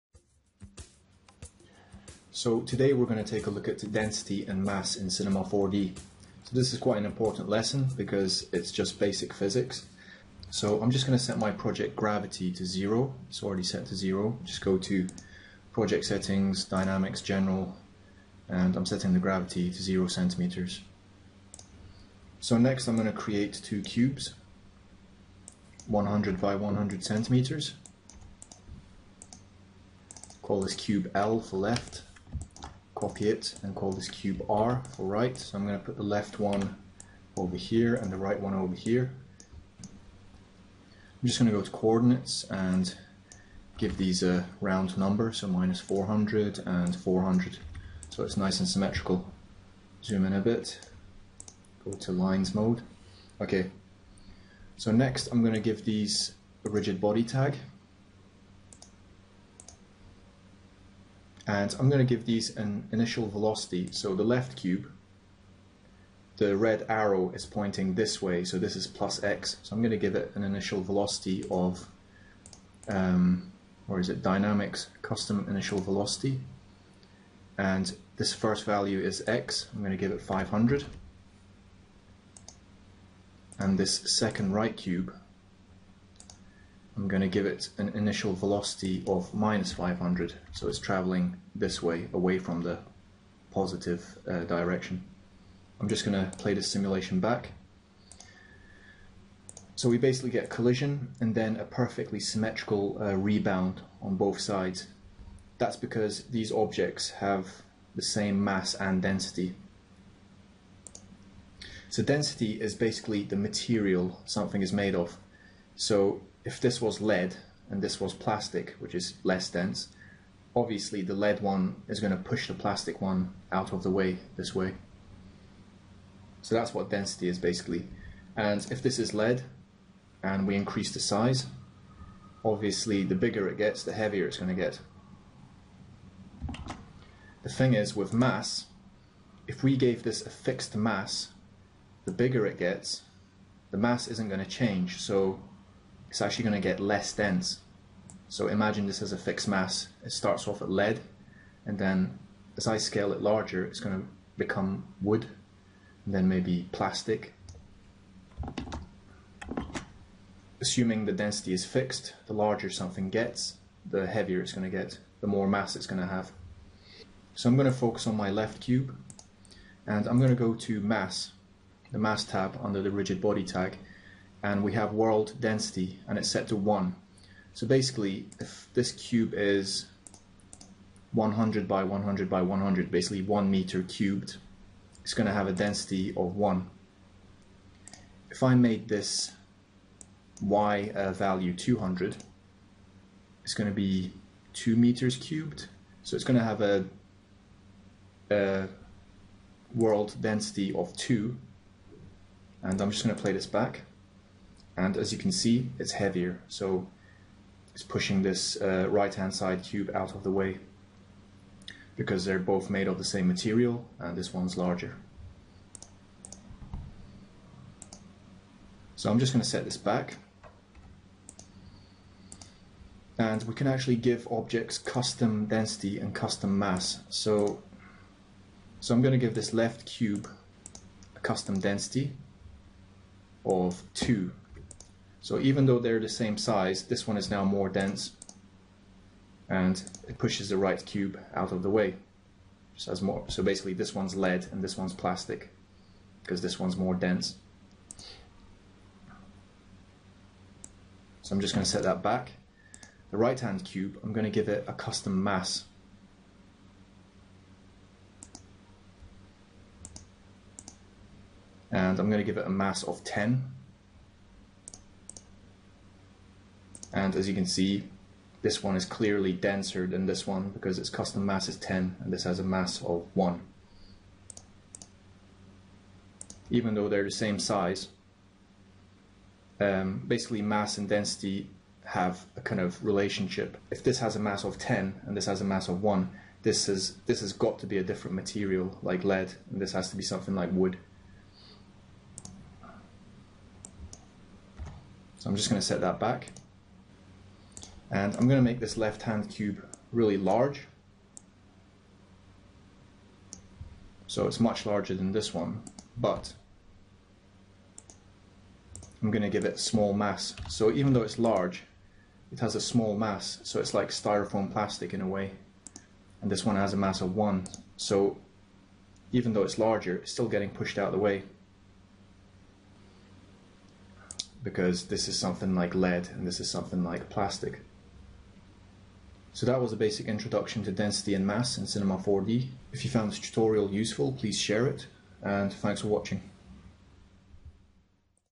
Cinema 4D Dynamics PART 12: What is Density and Mass?
If you found this tutorial useful, you can We take a look at what density and mass mean in Cinema 4D dynamics...